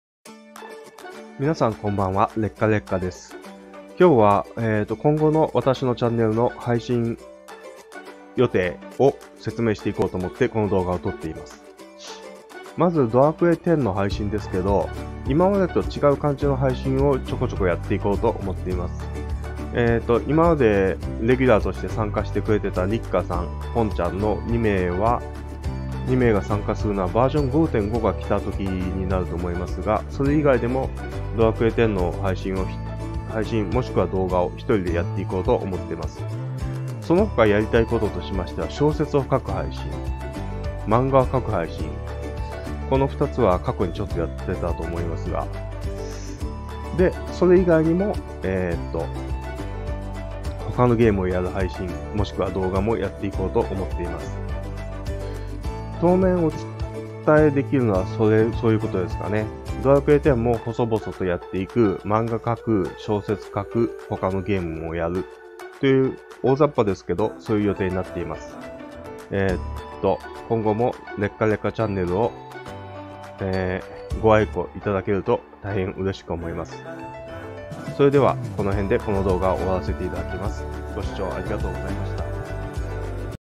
劣化烈火チャンネル今後の予定（2021年1月2日投稿）
劣化烈火チャンネルの今後の予定を大雑把に説明しています。

結論としては

・ドラクエ10（ソロプレイ？）
・小説を書く
・漫画を描く
・その他（ゲーム）

ということになります。